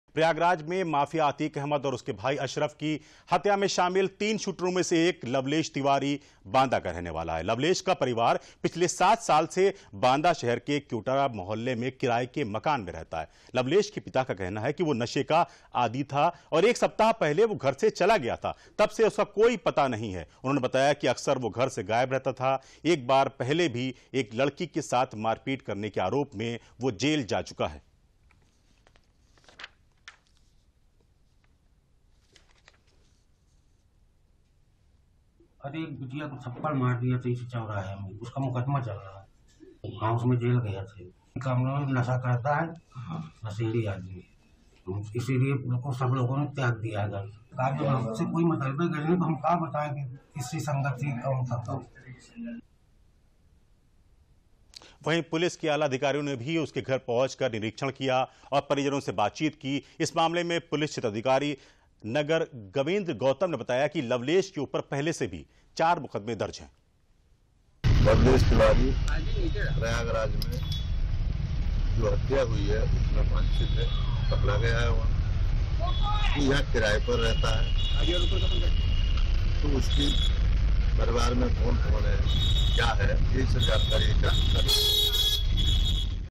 अतीक अहमद और अशरफ पर गोली चलाने वाला शूटर लवलेश तिवारी पहले भी जा चुका है जेल
प्रयागराज में माफिया अतीक अहमद और उसके भाई अशरफ की हत्‍या में शामिल तीन शूटरों में से एक लवलेश तिवारी बांदा का रहने वाला है। लवलेश का परिवार पिछले 7 साल से बांदा शहर के क्योटरा मोहल्ले में किराए के मकान में रहता है। लवलेश के पिता का कहना है कि वह नशे का आदी था और एक सप्ताह पहले वह घर से चला गया था, तब से उसका कोई पता नहीं है। उन्होंने बताया कि अक्सर वह घर से गायब रहता था और एक बार पहले भी एक लड़की के साथ मारपीट करने के आरोप में जेल जा चुका है।